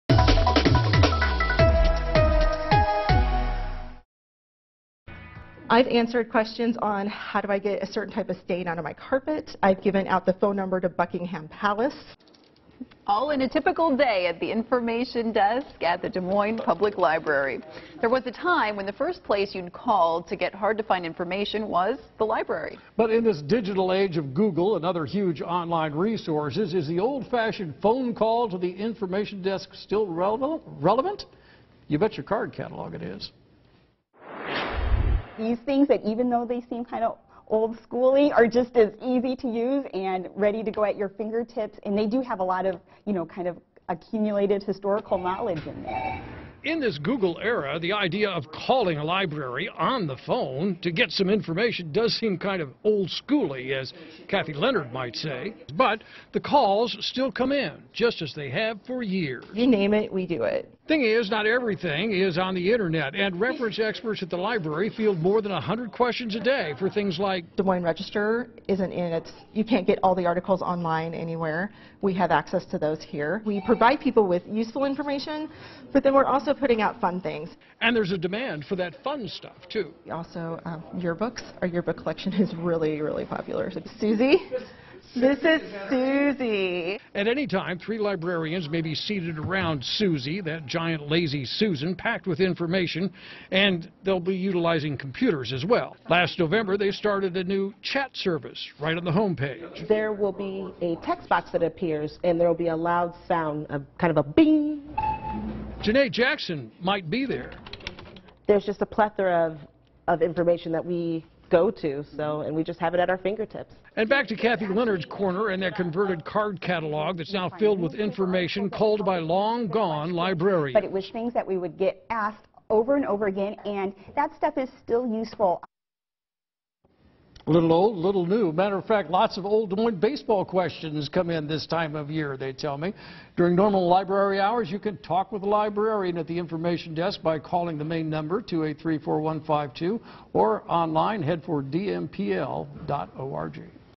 Actual humans still answer your questions at library
In an era of Google and the Internet, the Des Moines Public Library still answers questions you need answers to.